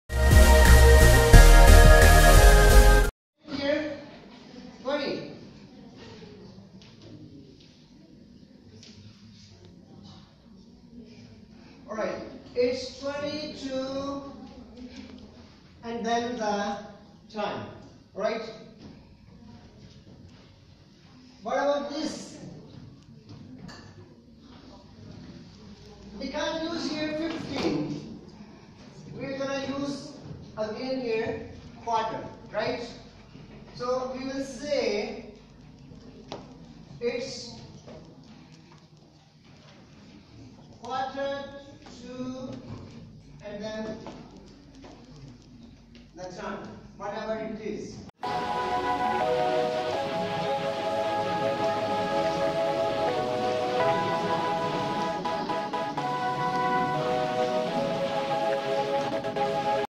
My Oral English Class in Shenyang Agricultural University China
Teach Oral English In China